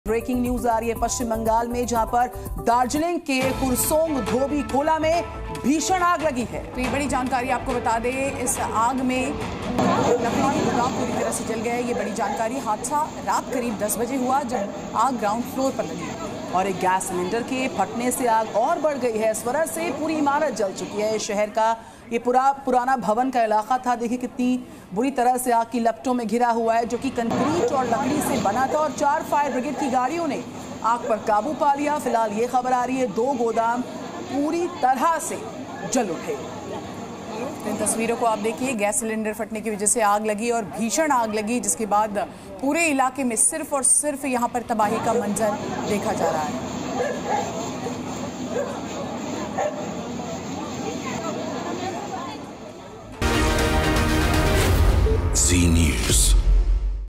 Breaking News: आग की लपटों से 'दहला' बंगाल | West Bengal Fire Incident | Latest Update | Hindi News
पश्चिम बंगाल में दार्जिलिंग के कूरसोंग धोबी खोला में भीषण आग लग गई है... जिसमें दो लकड़ी के गोदाम पूरी तरह से जल गए।हादसा रात करीब 10 बजे हुआ, जब आग ग्राउंड फ्लोर से शुरू हुई। एक गैस सिलेंडर के फटने से आग और बढ़ गई, जिसकी वजह से पूरी इमारत जल गई। ये शहर का पुराना भवन था, जो कंक्रीट और लकड़ी से बना था। चार फायर ब्रिगेड की गाड़ियों ने आग पर काबू पाया.

A massive fire broke out in Kursong Dhobi Khola in Darjeeling, West Bengal... in which two wooden warehouses were completely burnt. The incident happened around 10 pm when the fire started from the ground floor. The fire increased further due to the explosion of a gas cylinder, due to which the entire building burned down. This was an old building of the city, which was made of concrete and wood. Four fire brigade vehicles controlled the fire.